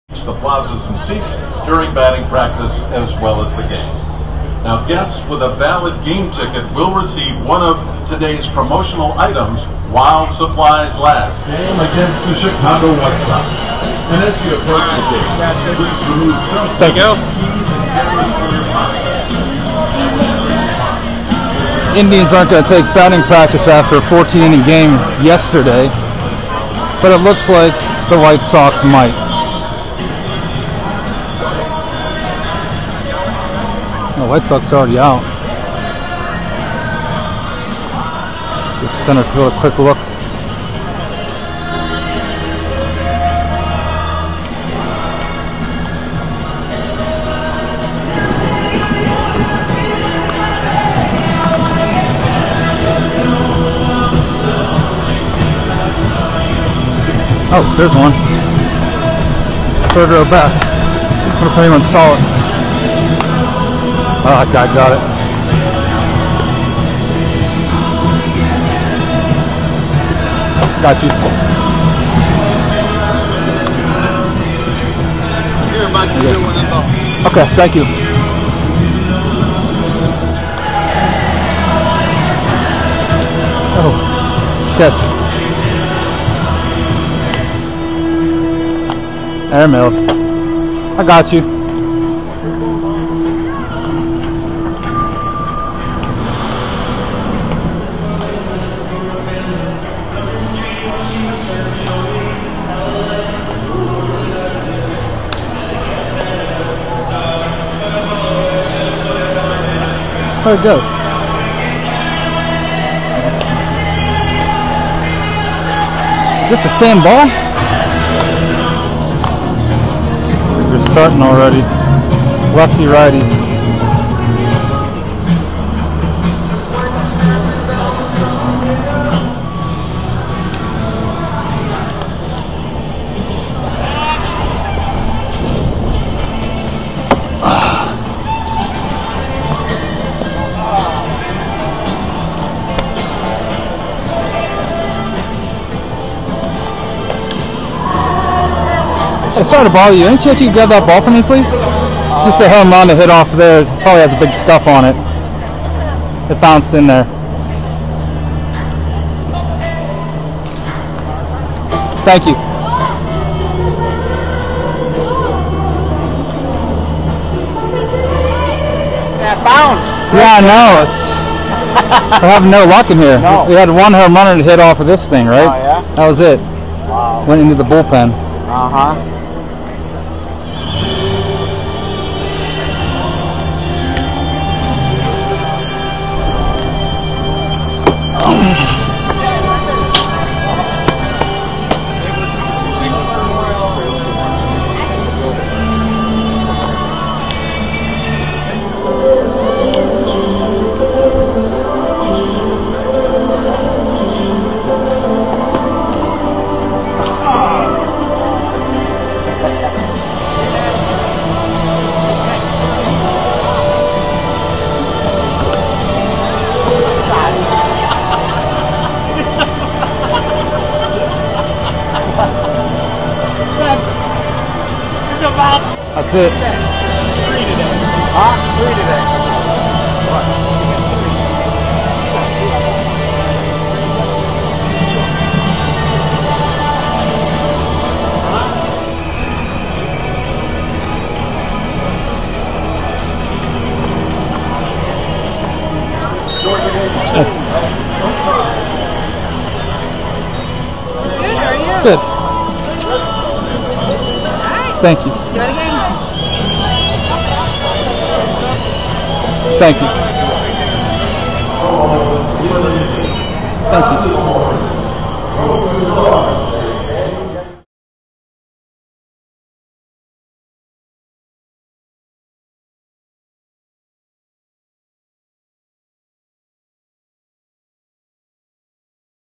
ROLE REVERSAL: TOSSING A PLAYER A BALL
Usually the players toss fans baseballs. But sometimes, the tables are turned. I helped out a White Sox player in his time of need on Memorial Day.